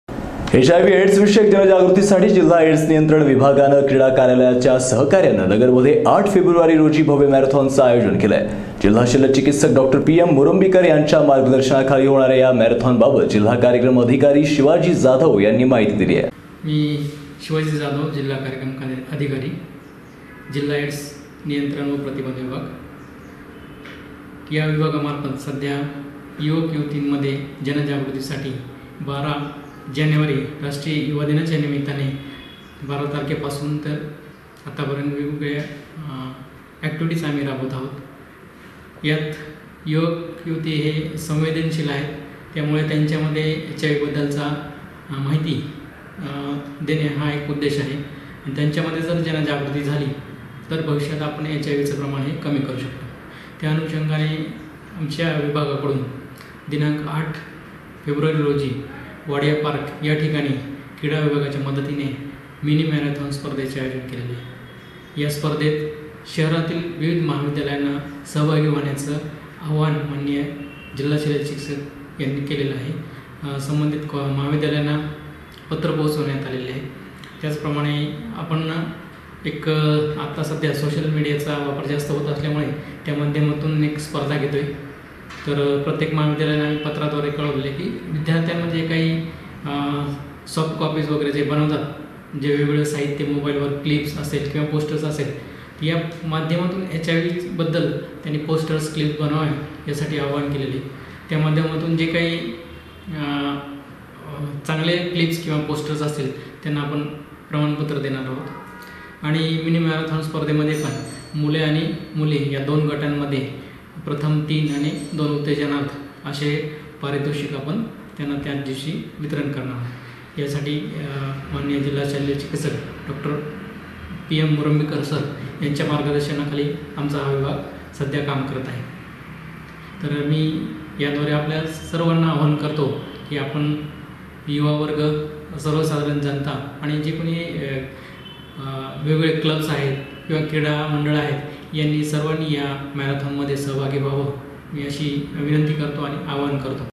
महानगर न्यूज - एचआयव्ही एडस्‌विषयक जनजागृतीसाठी 8 फेब्रुवारीला नगरमध्ये भव्य मॅरेथॉन
जिल्हा कार्यक्र्रम अधिकारी शिवाजी जाधव यांची माहिती